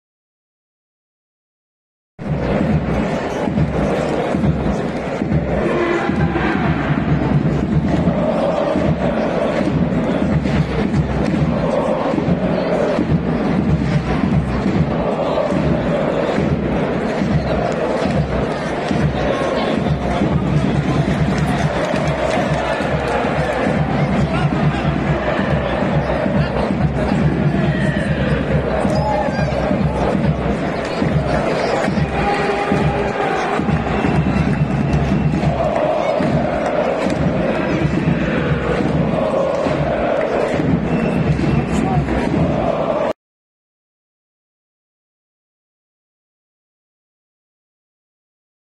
Kibice Warty Poznań w Białymstoku | 29.04.2023
► Ekstraklasa: Jagiellonia Białystok - Warta Poznań, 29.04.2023